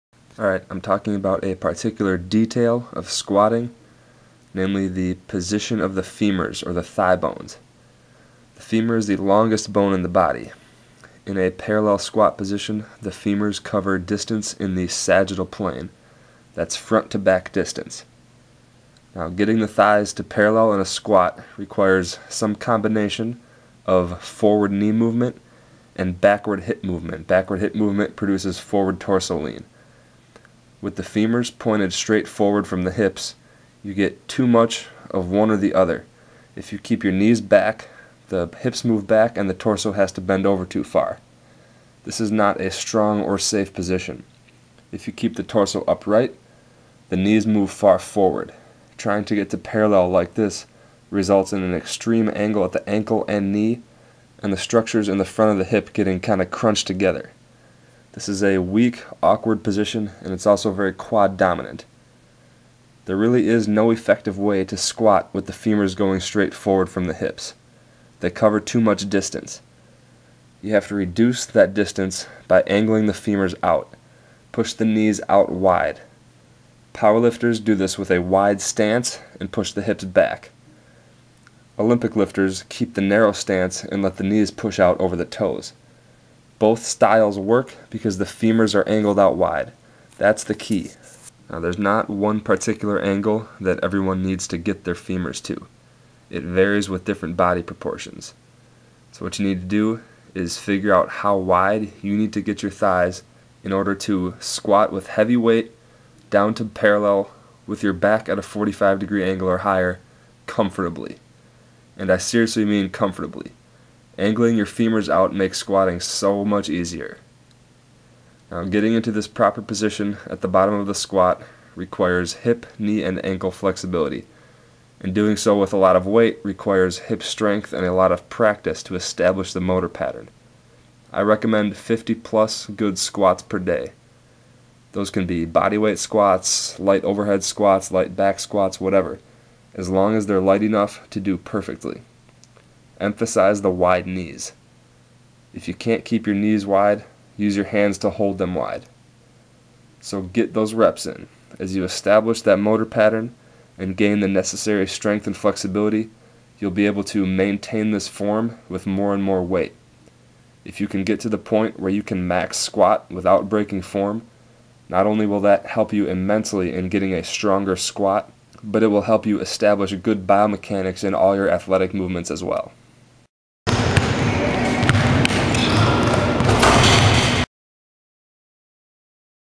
How to Squat Correctly: Femur Position
for free info on speed and jump training.

Squatting is a very hard movement to master. Very few people squat well, because most do not really know what a good squat position looks like. The typical instructions are something like "shoulder width stance, feet straight forward, push the hips back, sit into the squat, keep your knees over your feet, keep your chest up." That set of cues DOES NOT WORK! I'm doing my best to set people straight on squat technique.